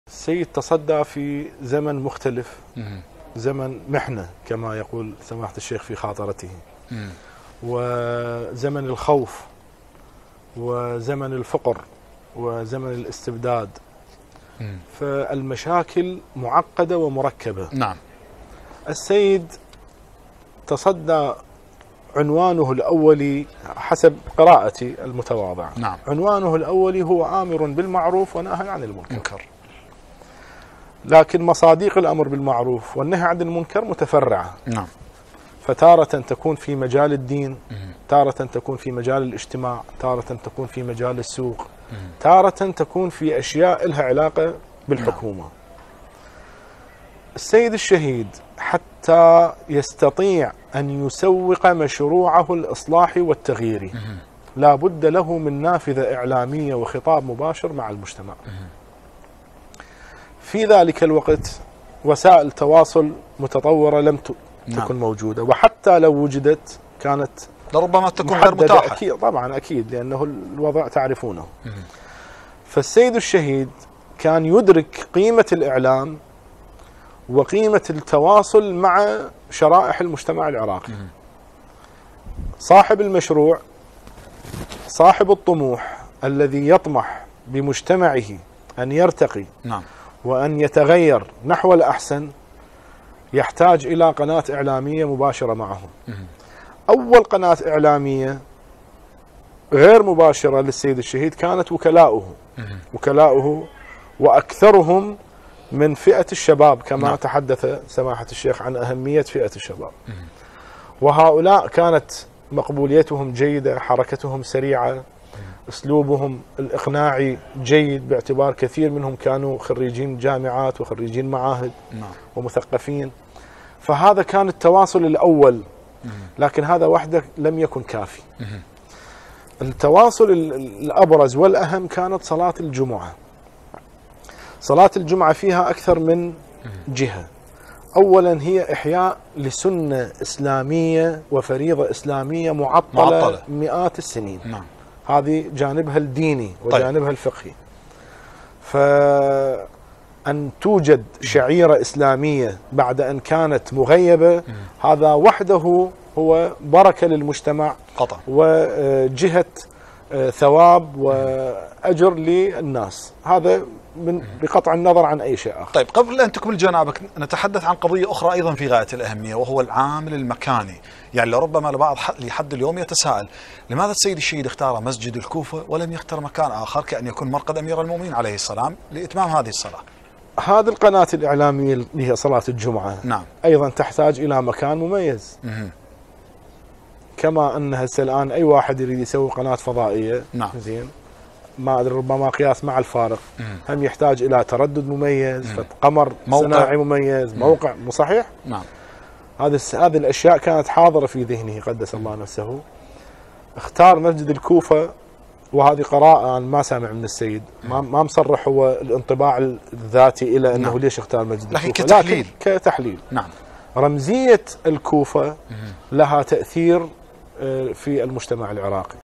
الملخص - كاظم الحسيني - صلاة الجمعة كانت القناة الاعلامية للشهيد الصدر
الملخص مع احمد العذاري الشهيد الصدر.. ذلك الذي ارعب الطغاة و هز عروشهم
د. صاحب نصار / باحث اسلامي
كاظم الحسيني/ استاذ الحوزة العلمية في النجف الاشرف